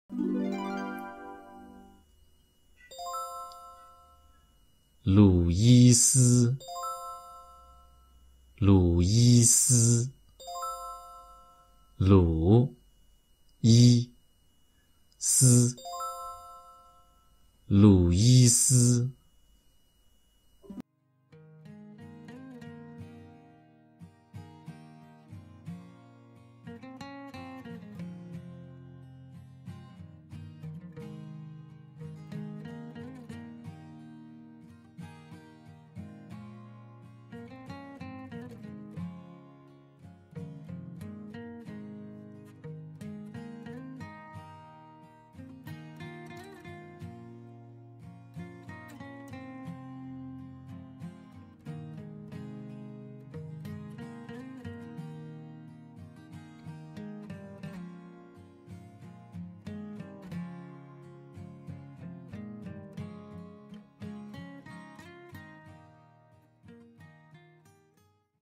How to Say Your Name RUIZ in Chinese?
*Formal* Chinese translation of your name RUIZ. This video shows how native Chinese speakers pronounce and write it (the stroke sequence in Chinese).

-- topic suggestion
-- constructive feedback
-- quick questions